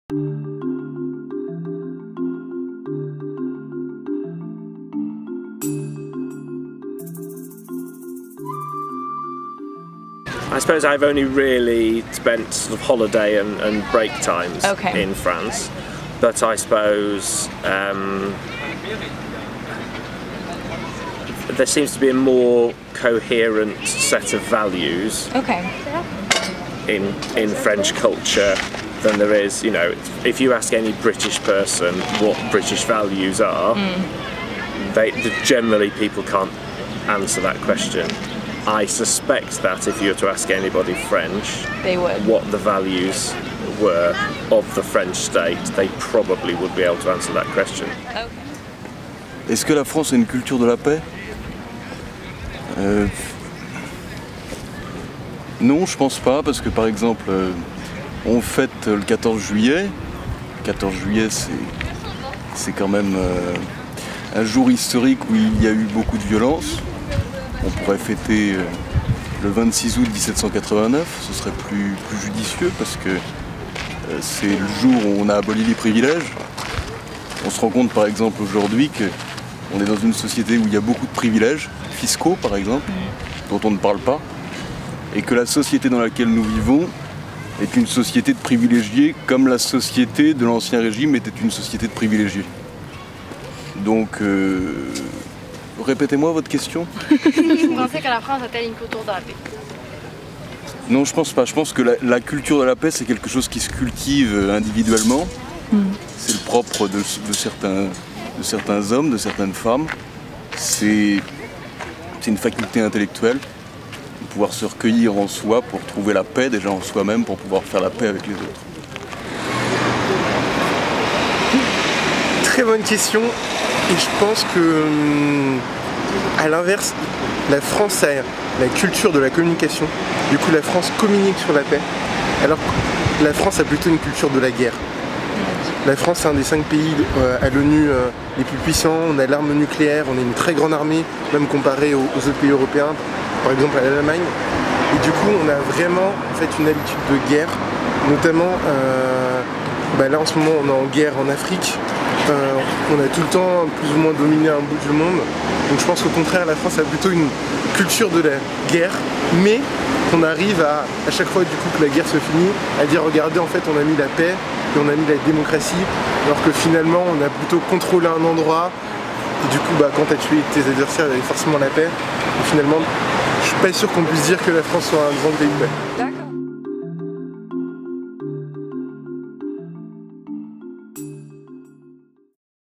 La culture de la paix en France
Notre pays a t-il "la culture de la Paix" ? Nous avons posé la question à des citoyens et citoyennes...